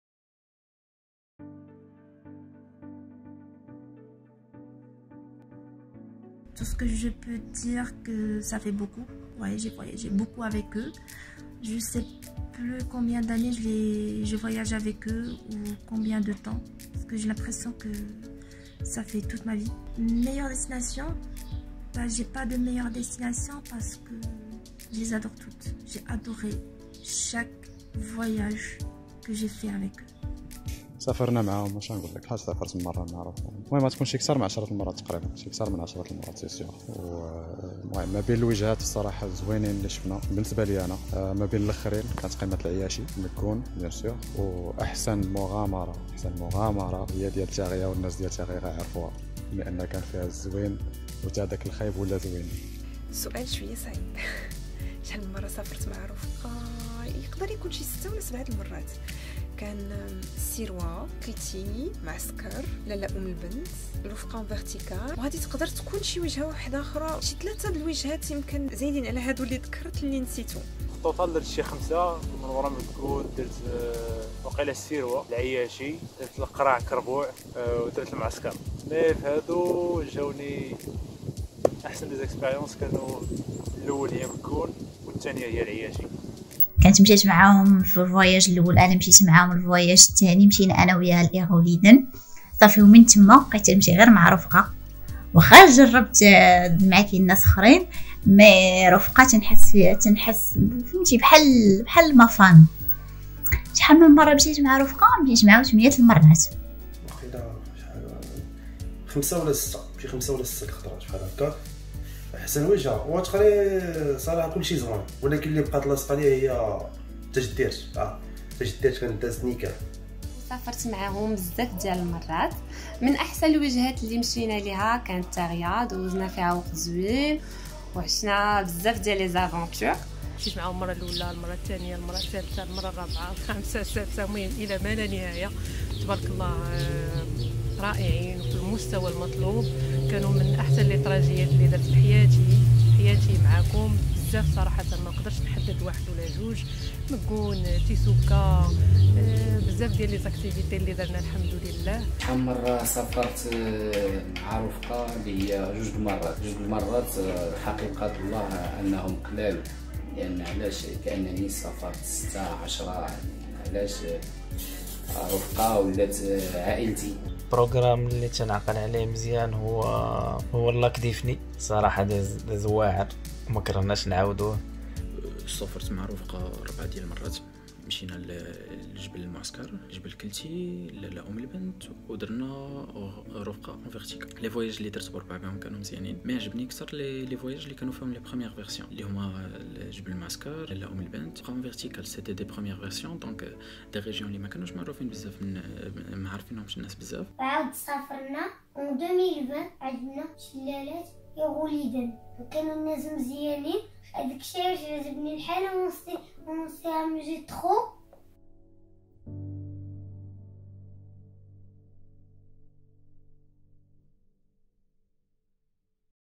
شحال من مرة سافرتي معانا ؟ شنو هي أحسن وجهة درتيها معانا ؟
دائما في إطار أننا توحشناكم 😊 كان ضروري نشوفوكم او نسمعوكم ، لذلك كانو عندنا بعض الاسئلة.
سؤال اليوم :
_ شحال من مرة سافرتي معانا ؟
_ شنو هي أحسن وجهة درتيها معانا ؟